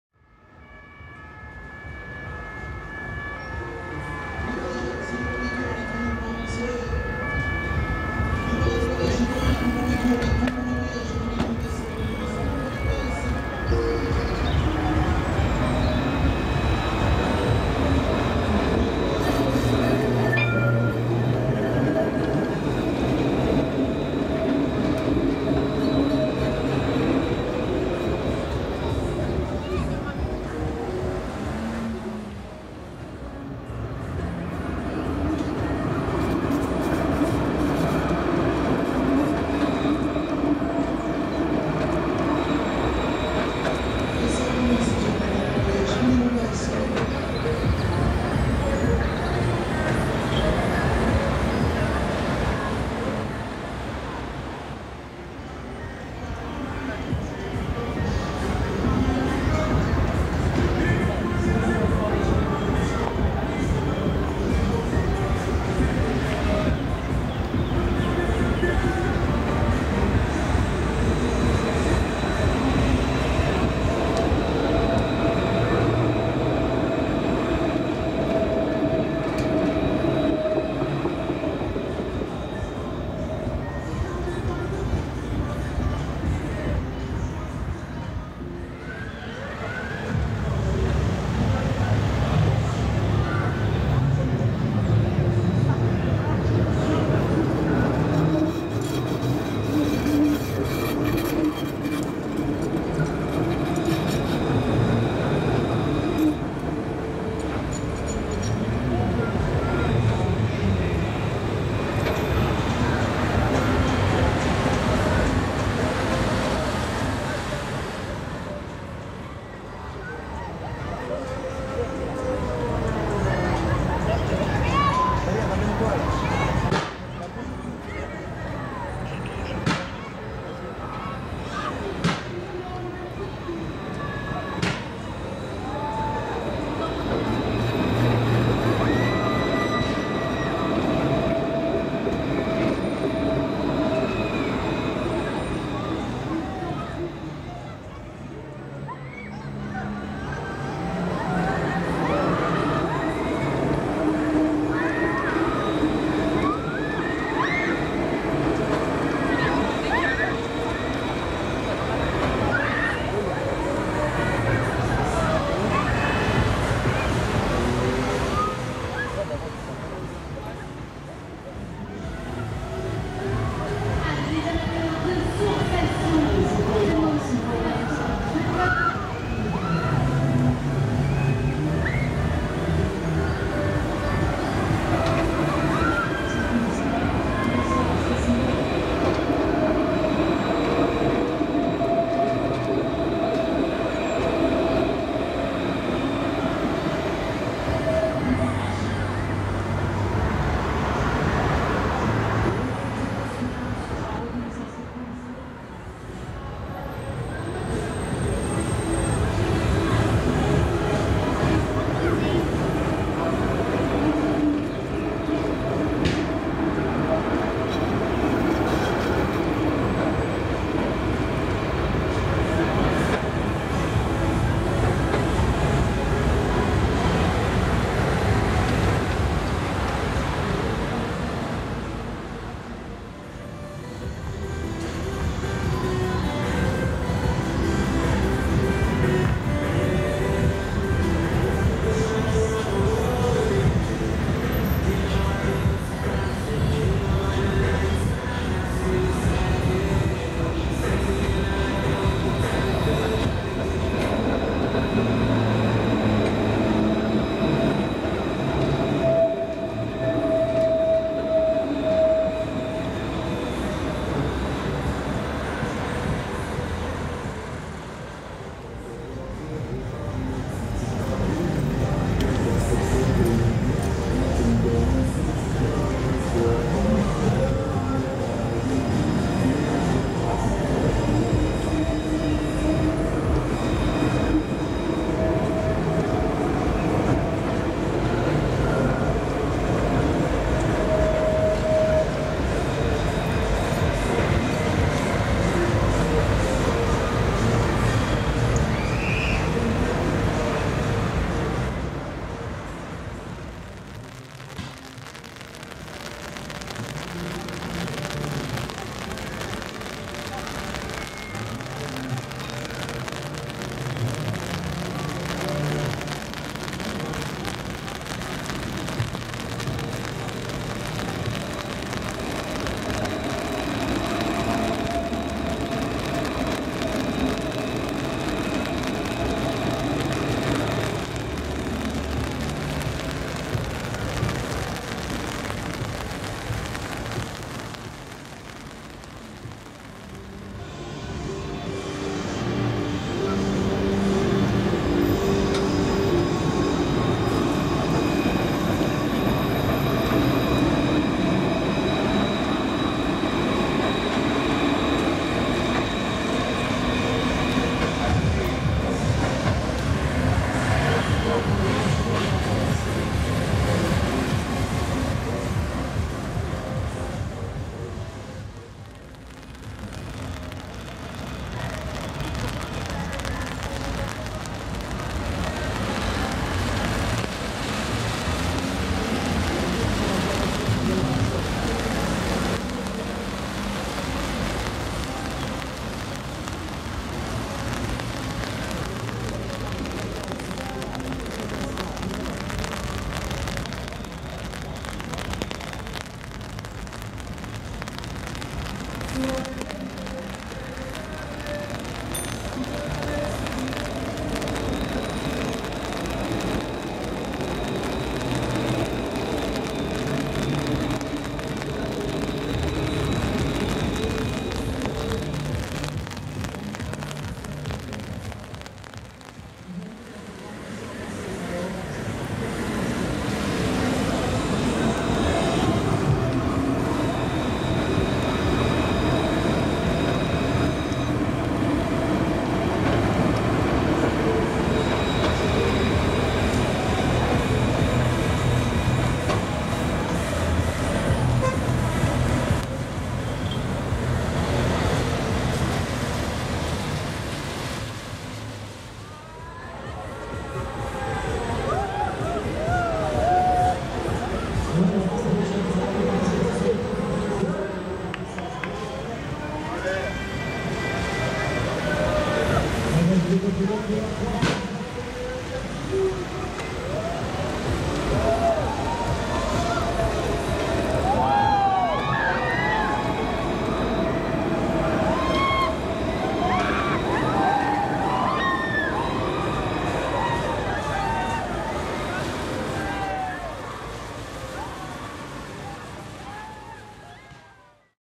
Des Citadis à la Fête Foraine - Foire des Rameaux 2024 Grenoble
Cette année certains jours de week-ends la ligne E est exploitée en matériel Citadis pour la forte affluence causée par la foire des rameaux.

L’occasion de voir pour la première fois des rames Citadis traversé l’esplanade de Grenoble avec les manèges de la foire.